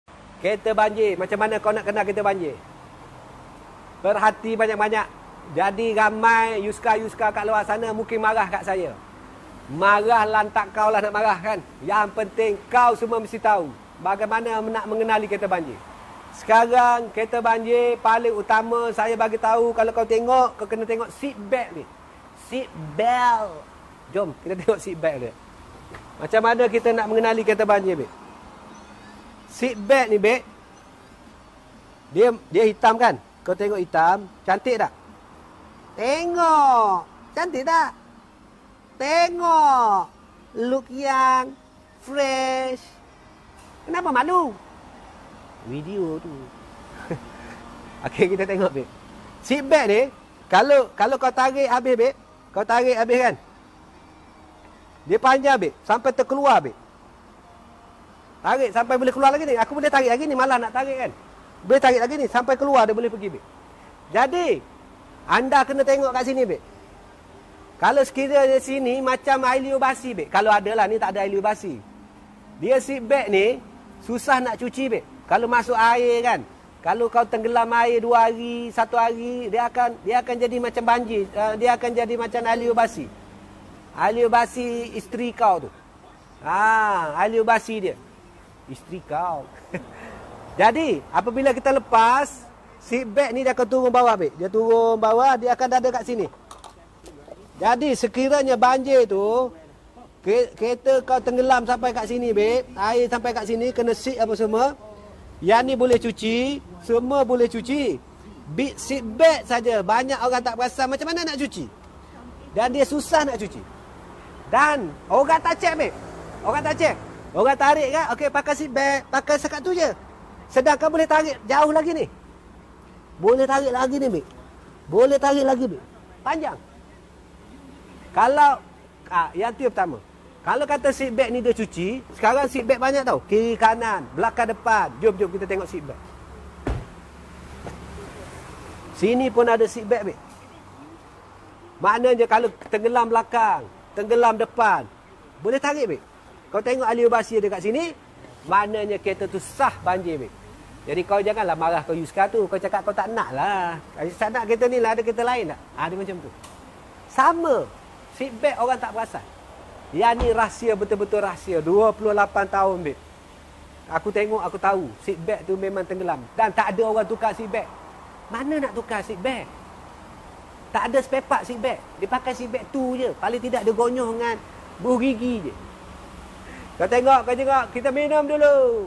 PecahRahsia 𝗧𝗜𝗣𝗦 𝗠𝗨𝗗𝗔𝗛 𝗠𝗘𝗡𝗚𝗘𝗦𝗔𝗡 𝗞𝗘𝗥𝗘𝗧𝗔 𝗧𝗘𝗡𝗚𝗚𝗘𝗟𝗔𝗠 𝗕𝗔𝗡𝗝𝗜𝗥!!
𝗞𝗢𝗬𝗔𝗞 𝗕𝗘𝗕 𝗞𝗔𝗟𝗔𝗨 𝗞𝗢 𝗧𝗘𝗥𝗕𝗘𝗟𝗜 𝗞𝗘𝗥𝗘𝗧𝗔 𝗧𝗘𝗡𝗚𝗚𝗘𝗟𝗔𝗠 𝗕𝗔𝗡𝗝𝗜𝗥!!
Jom.. jom kau tengok kau jengok
𝗧𝗜𝗣𝗦 𝗠𝗨𝗗𝗔𝗛 𝗠𝗘𝗡𝗚𝗘𝗦𝗔𝗡 𝗞𝗘𝗥𝗘𝗧𝗔 𝗧𝗘𝗡𝗚𝗚𝗘𝗟𝗔𝗠 𝗕𝗔𝗡𝗝𝗜𝗥!!
Weyyy.. kesan air lumpur, selut semua tu boleh kawtim.. boleh kasi cuci bersih tapiii... bahagian nie diaorang (penjual) dia xbuat...
𝗖𝗗𝗞𝗔 𝗞𝗔𝗦𝗜 𝗧𝗔𝗪...

✅IC BOTHSIDE
✅LESEN BOTHSIDE

Sebarang kemusykilan,
sila hubungi